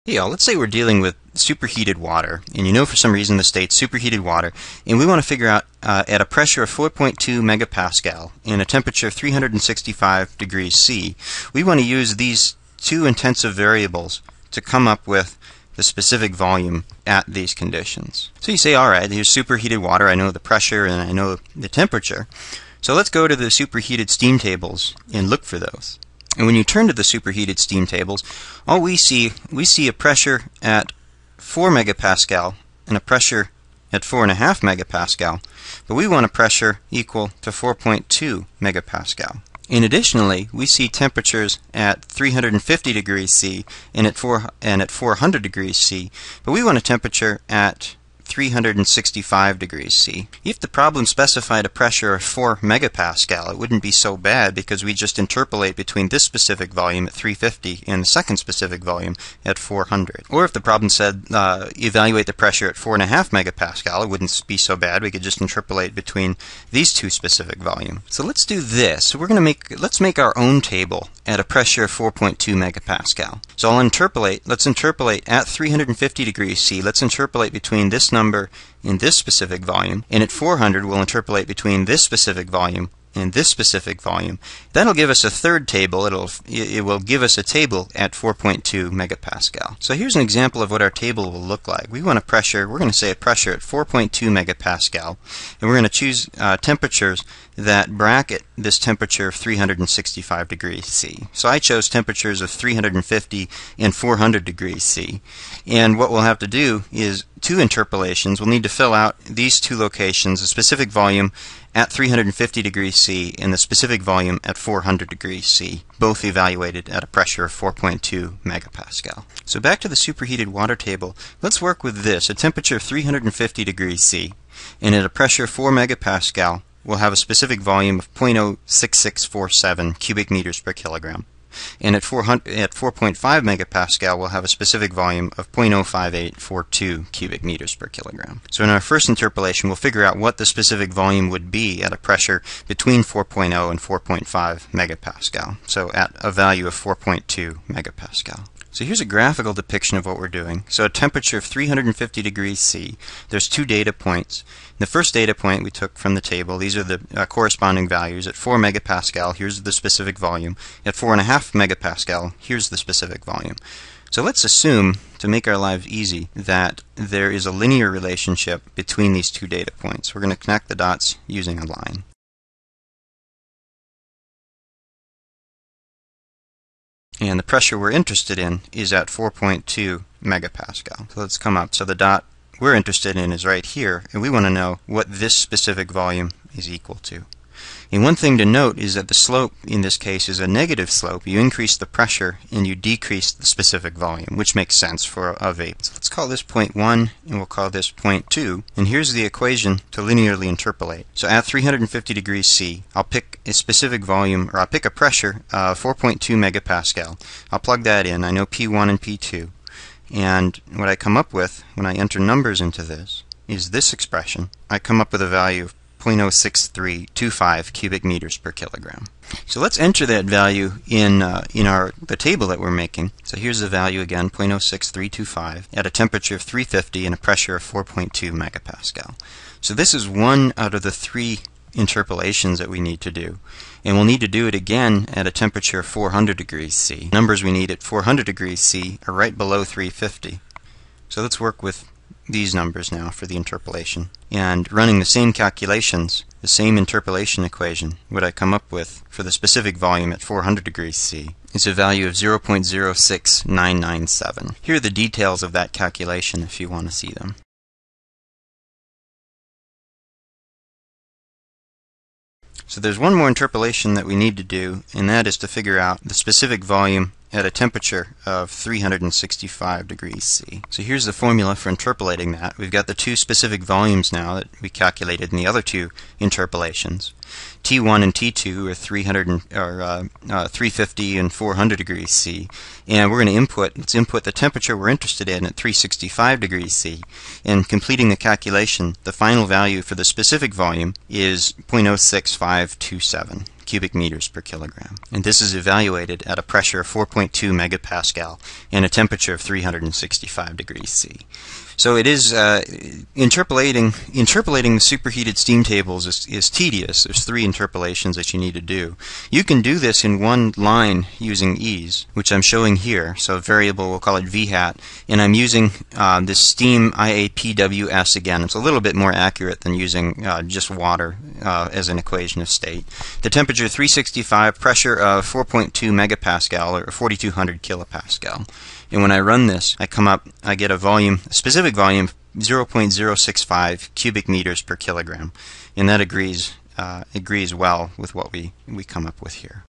Interpolation 2
Interpolating the superheated water tables to find the specific volume at a specified temperature and pressure